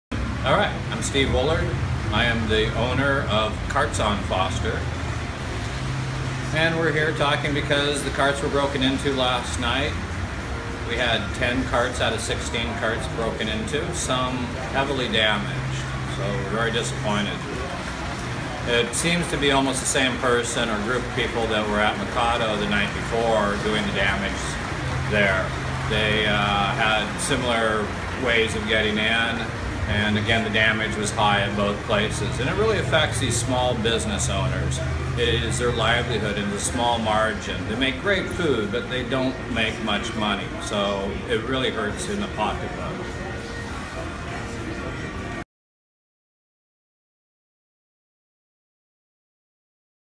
Carts on Foster owner talks about break-ins
In this video, Carts on Foster owner Steve Woolard speaks about damage done to ten food carts at his Southeast 52nd Avenue and Foster Street location.